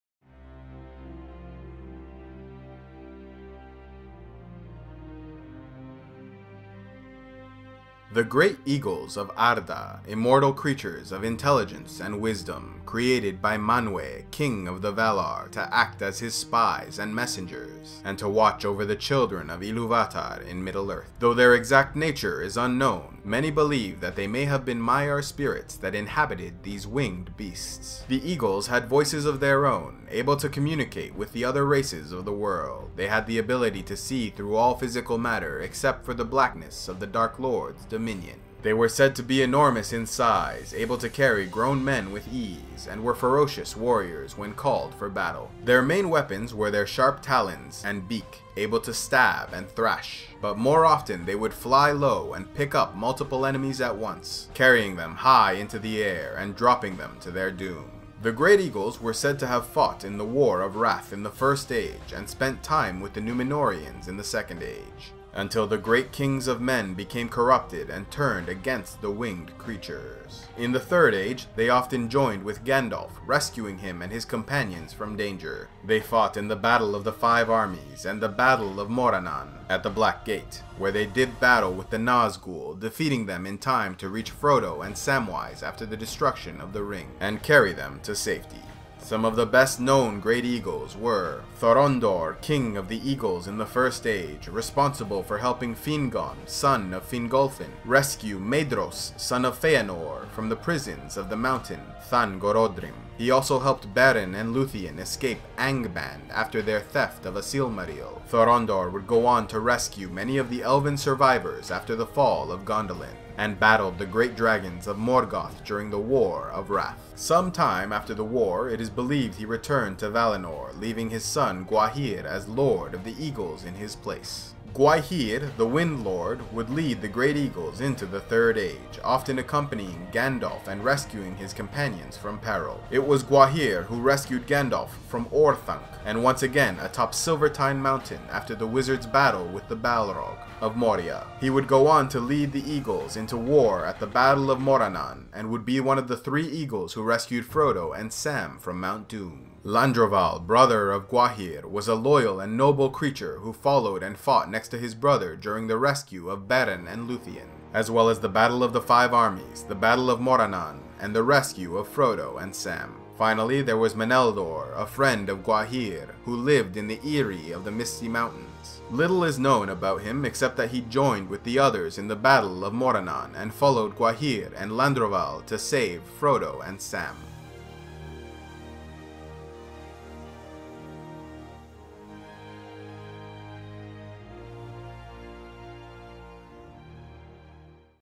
Focus: Eagles (LOTR)
A Brief History of the Eagles. Based on the "Lord of the Ring" Series as well as the expanded Universe Writings by J. R. R. Tolkien.

Overview

Creation

First Age

Second Age

Third Age

Battles

Thorondor

Gwaihir

Landroval

Meneldor